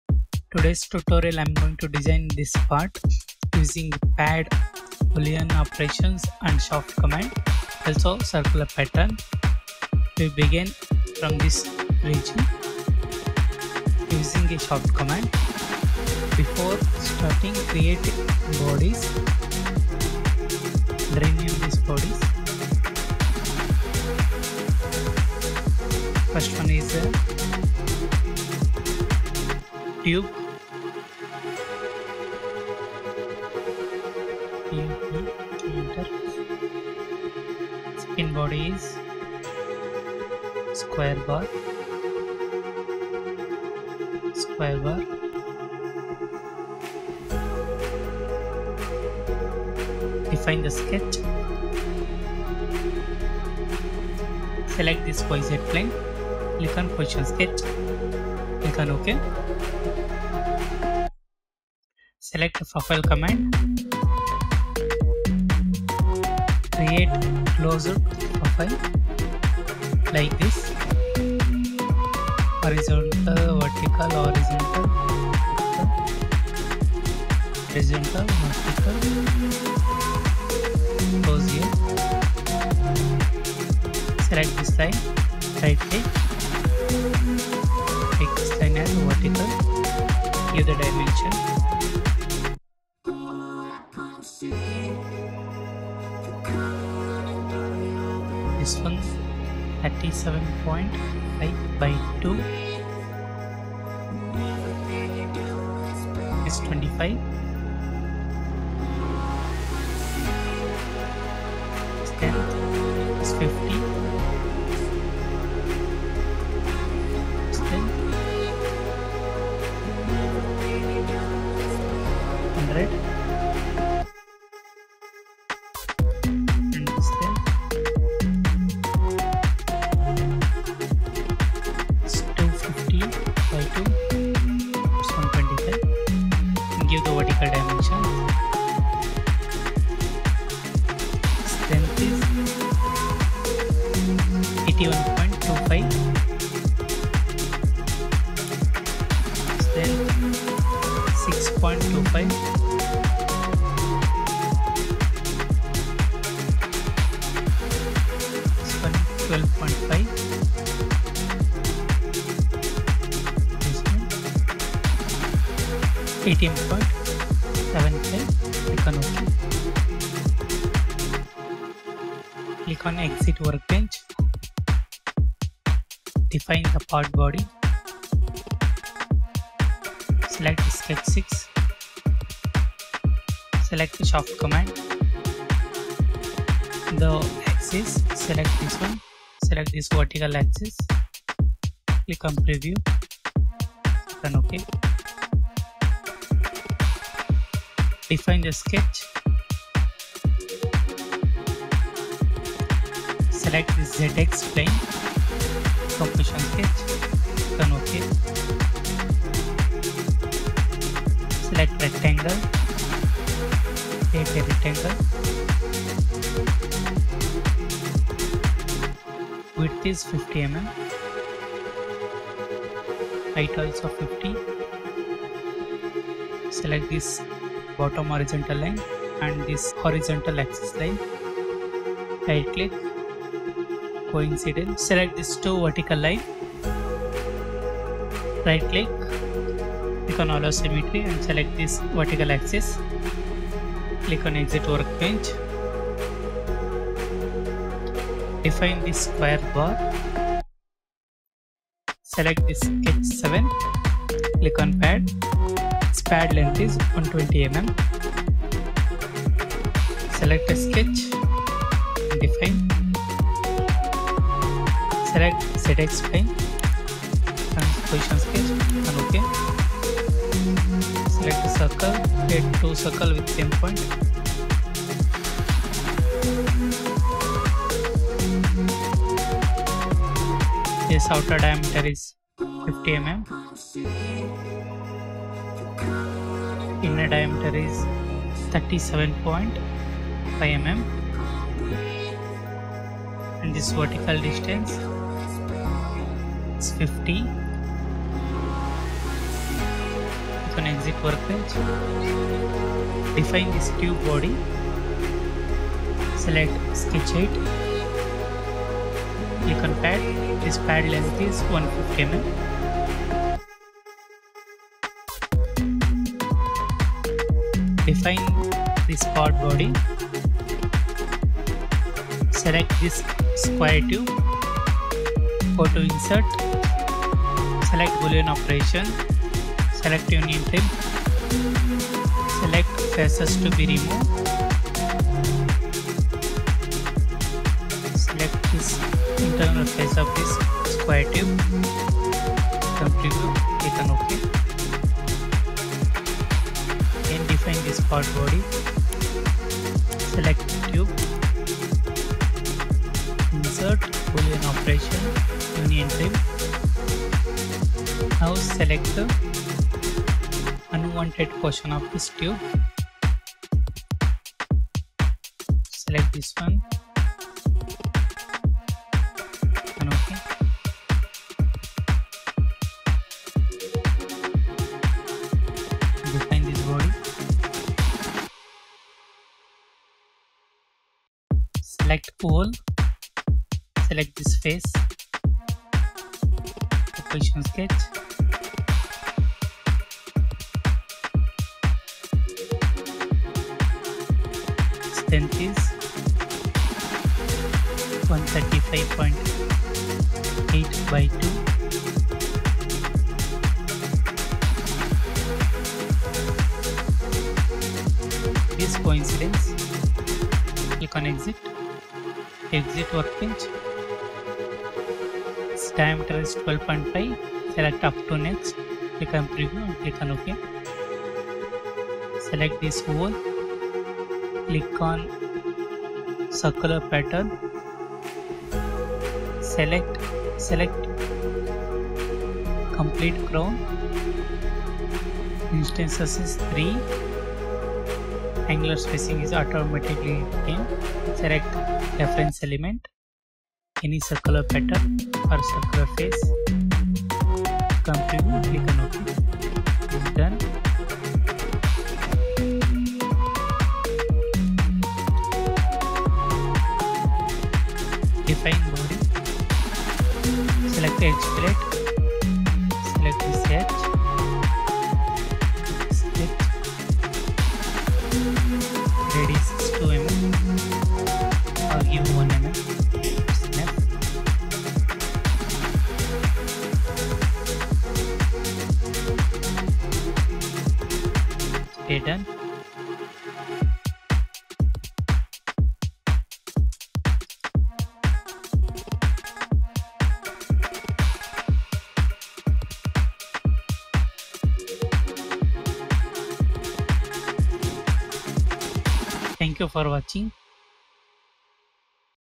CATIA V5 Tutorial: Master Part Design with Sketch, Shaft, Pad, New Body, Circular Pattern, & Boolean
Welcome to The Product Designer.

Learn how to create a complex part with intricate features using CATIA V5's Part Design workbench. This tutorial will guide you through designing Part 24, where you'll utilize the Sketch, Shaft, Pad, New Body, Circular Pattern, and Boolean commands to create a detailed part. Follow along step-by-step as we demonstrate the techniques involved in this exercise.

In this tutorial, you'll learn:

* How to create a complex part from scratch using basic sketching techniques.
* How to add shaft features to your part using the Shaft command.
* How to add solid features to your part using the Pad command.
* How to create a new body within your part using the New Body command.
* How to efficiently replicate features using the Circular Pattern command.
* How to combine or subtract features using Boolean operations.
* How to refine your part's details using drafting tools.

By the end of this tutorial, you'll have a solid understanding of the key features and techniques used in CATIA V5's Part Design workbench. You'll be able to apply these skills to create a variety of complex parts for your projects, such as mechanical components, assemblies, and more.

I appreciate your feedback and look forward to bringing you more exciting content in the future.